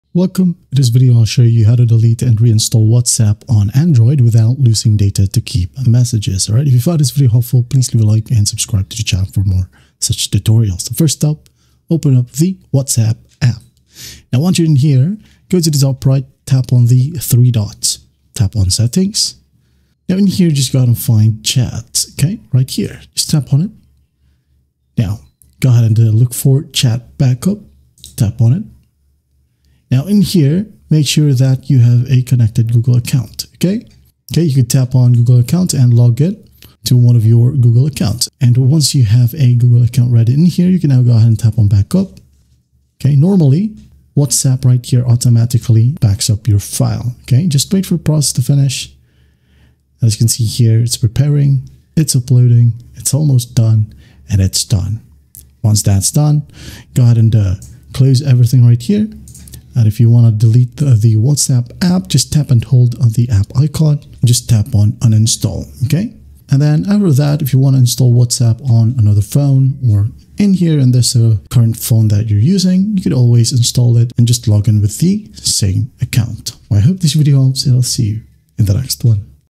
How To Delete And Reinstall WhatsApp On Android Without Losing Data To Keep Messages (Full Guide)
How To Delete And Reinstall WhatsApp On Android Without Losing Data To Keep Messages
✅ Get Netflix, Disney+, ChatGPT, Spotify Premium and more much cheaper with Gamsgo

In this video, I will show you how to delete and reinstall WhatsApp on Android without losing your chats. Many users worry that uninstalling WhatsApp may erase their data, but with proper backup settings, you can keep all your messages. I’ll guide you through backing up your chats to Google Drive, uninstalling WhatsApp, reinstalling it, and restoring the backup. By the end, you’ll know how to reinstall WhatsApp on Android safely while keeping your messages intact.

Chapters:
00:00 Introduction
00:10 How To Delete And Reinstall WhatsApp On Android Without Losing Data To Keep Messages